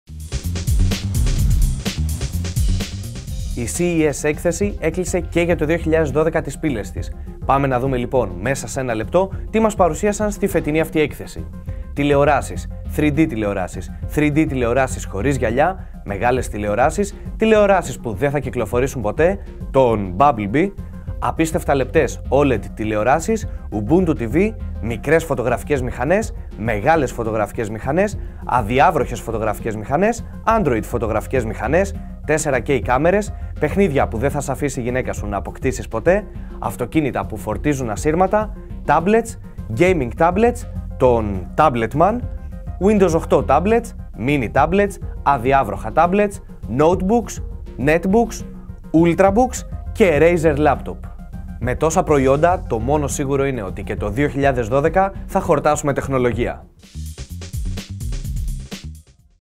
Η CES Έκθεση 2012 σε 60"!
Το Getitnow παει... LAS VEGAS! Όλα οσα ειδαμε στην φετινη εκθεση, μεσα σε 60". 3D TVS, 4k καμερες, αδιαβροχα tablets κ.α! Ποιο ξεχωρισατε εσεις???